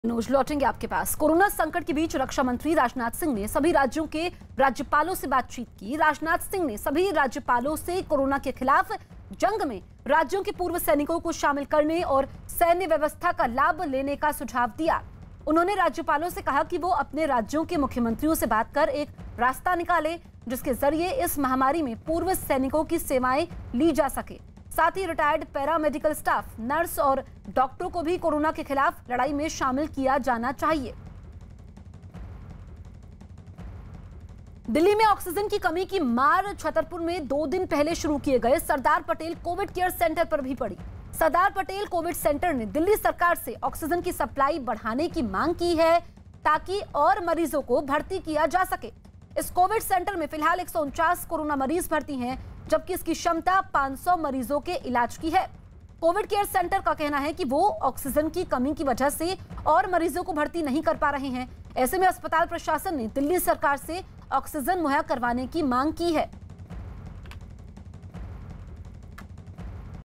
Rajnath Singh की सभी राज्यपालों से Corona पर चर्चा, पूर्व सैनिकों और डॉक्टरों से मदद पर दी राय
भारत में कोरोना वायरस की दूसरी लहर से चारों तरफ हाहाकार (Coronavirus Second Wave) मचा हुआ है. रोजाना सैकड़ों की संख्या में संक्रमित लोगों की मौत हो रही है. कोरोना के बिगड़े हालातों को देखते हुए रक्षा मंत्री राजनाथ सिंह ने मंगलवार को सभी राज्य के राज्यपालों से बातचीत (Rajnath Singh meeting with governors) की. उन्होंने राज्यपालों से राज्य सरकारों को विश्वास में लेने के लिए कहा है.राजनाथ सिंह ने राज्यपालों से कहा कि वो अपने राज्यों के मुख्यमंत्रियों से बात कर एक रास्ता निकालें जिसके तहत इस महामारी में पूर्व सैनिकों की सेवाएं ली जा सकें. इसके अलावा, अर्धसैनिक कर्मचारियों, नर्सों और डॉक्टरों को जो फोर्सेस से सेवानिवृत्त हुए हैं, उन्हें भी कोरोना के खिलाफ लड़ाई में शामिल किया जाना चाहिए.